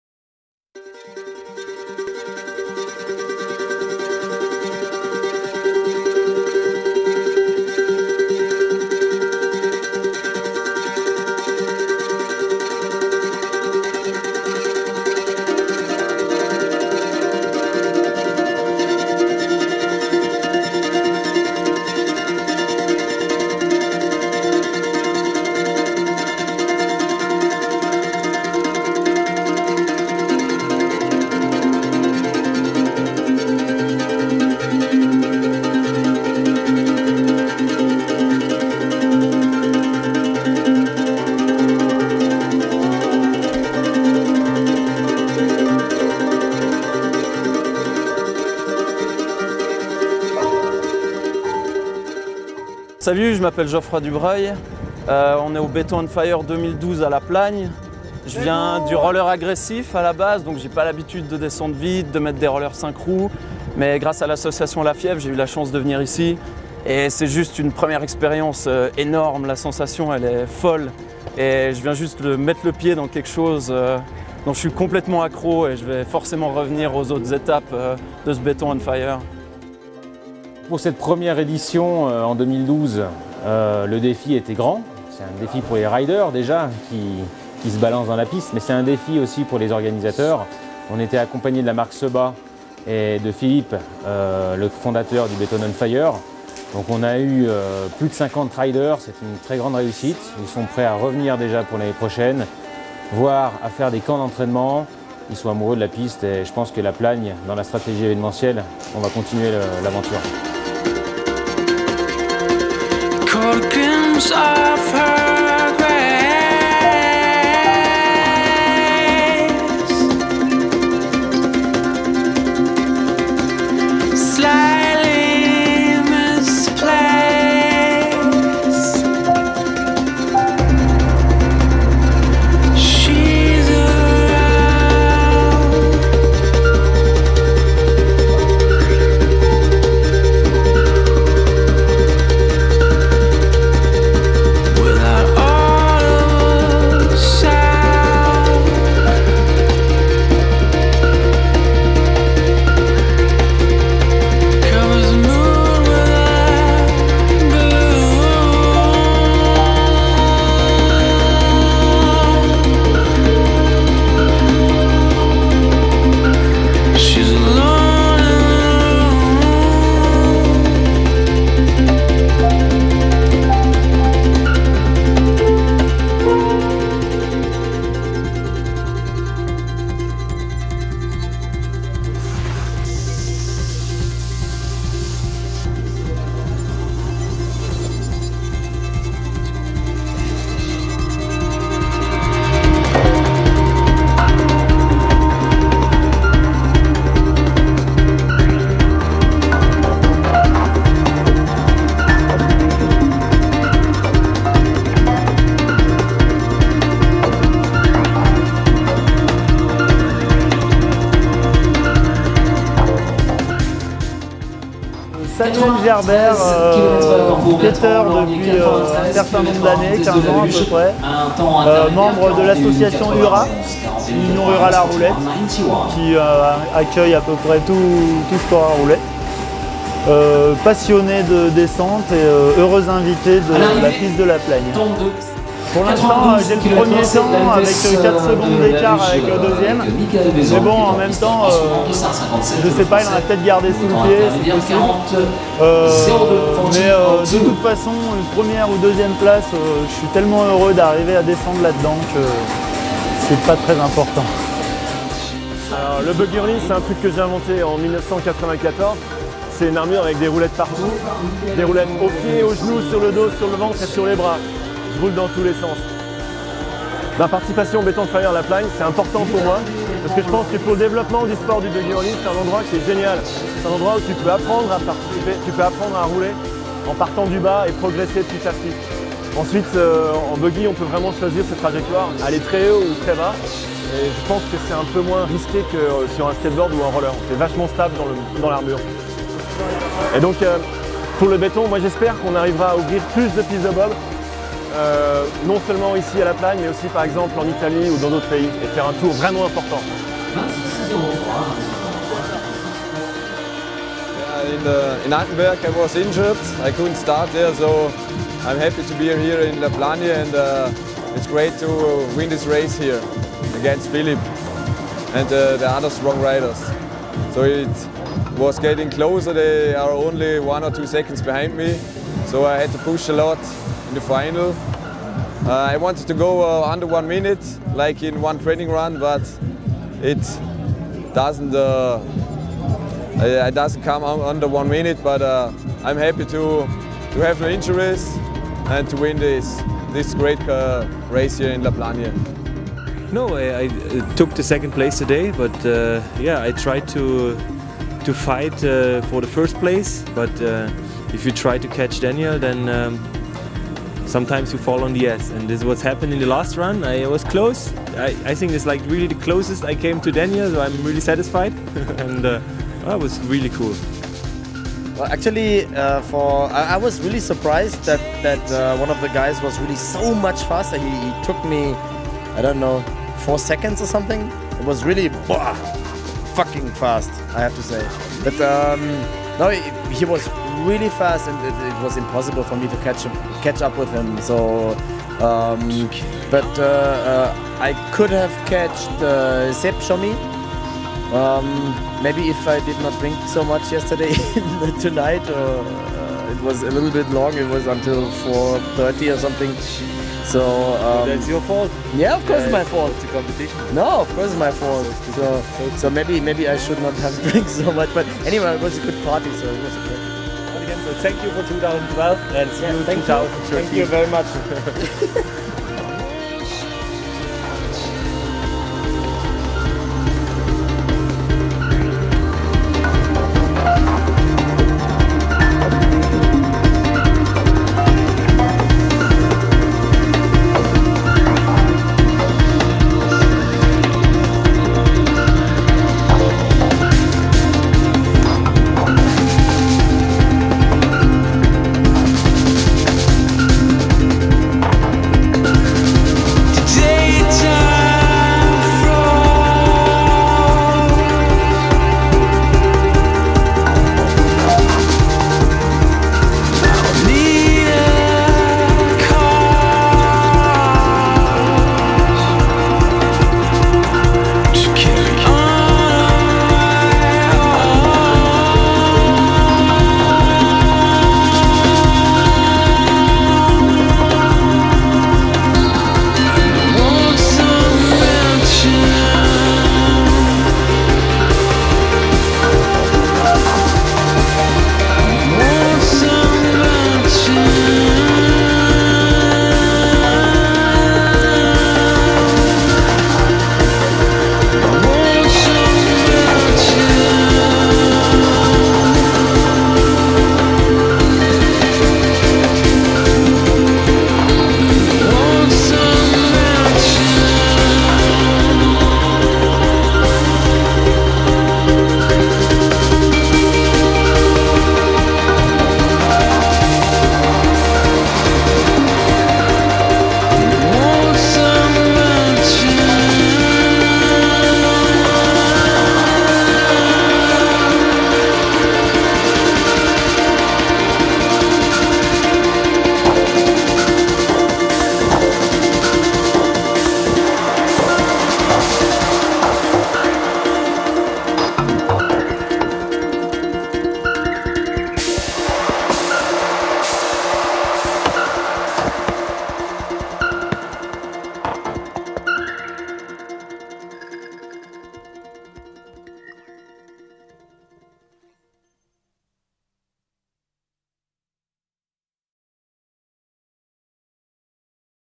SEBA Beton On Fire 2012 ' LA PLAGNE '
Results of the final in La Plagne :
Inline:
1st Daniel Ladurner AUT 1'00''220
2nd Philipp Auerswald GER 1'02''180
3rd Romain Guignet SUI 1'02''550
Longboard:
1st Samuel Gerber FRA 1'09''500
2nd Sébastien Chommy FRA 1'13''050
3rd Danny Strasser GER 1'18''630
Final results of the SEBA Beton on fire TOUR (Total points of Altenberg + La Plagne):
Longboard:
1st Danny Strasser GER (500 points)
2nd Sébastien Chommy FRA (485 points)
3rd Benjamin Haas GER (454 points)
Inline:
1st Philipp GER Auserwald (569 points)
2nd Roman Guignet SUI (519 points)
3rd Nicolas FRA VARIN (476 points)
Congratulations to all the competitors for their participation and see you in 2013 !
Seen on TV
- La Plagne TV
- TV8 Mont Blanc
- TéléMatin
- ZDF Germany TV
Beton On Fire
La plagne
Seba Skates
Red Bull
Rollerenligne
MCS Extreme
Waxx
FILM & PICTURES:
Anthony FINOCCHIARO
(Thanks to Jean-Yves Blondeau, Falko Quinger, Daniel Ladurner, Moritz norl and Luge & Longboarder for the additional GoPro footage..)
FULL ARTICLE AVAILABLE ON SEBANEWS.COM